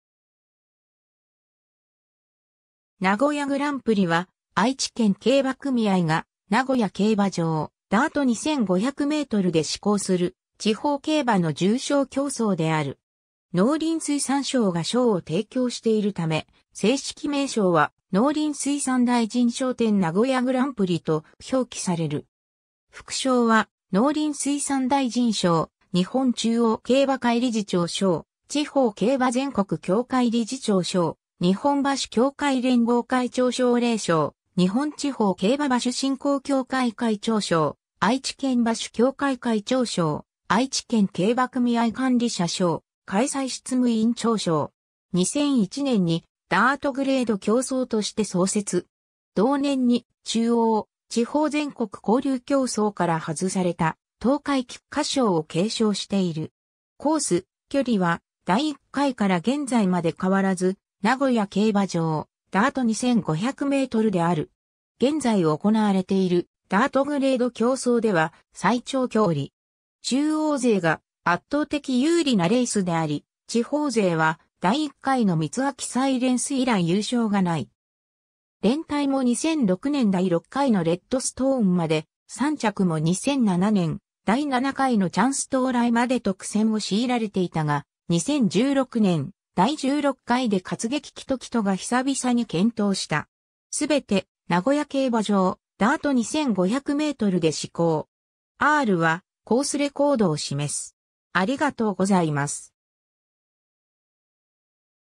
名古屋グランプリ
名古屋グランプリ（なごやグランプリ）は、愛知県競馬組合が名古屋競馬場ダート2500mで施行する地方競馬の重賞競走（ダートグレード競走、JpnII）である。
農林水産省が賞を提供しているため、正式名称は農林水産大臣賞典 名古屋グランプリと表記される。
 副賞は農林水産大臣賞、日本中央競馬会理事長賞、地方競馬全国協会理事長賞、日本馬主協会連合会長奨励賞、（一社）日本地方競馬馬主振興協会会長賞、（一社）愛知県馬主協会会長賞、愛知県競馬組合管理者賞、開催執務委員長賞（2020年）。
 2001年にダートグレード競走として創設。
同年に中央・地方全国交流競走から外された東海菊花賞を継承している。
コース・距離は第1回から現在まで変わらず、名古屋競馬場ダート2500mである。
 現在行われているダートグレード競走では最長距離。
 中央勢が圧倒的有利なレースであり、地方勢は第1回のミツアキサイレンス（笠松）以来優勝がない。
連対も2006年第6回のレッドストーン（地元・愛知）まで、3着も2007年第7回のチャンストウライ（兵庫）までと苦戦を強いられていたが、2016年第16回でカツゲキキトキト（地元・愛知）が久々に健闘した。
 全て名古屋競馬場ダート2500mで施行。
 ※ Rはコースレコードを示す。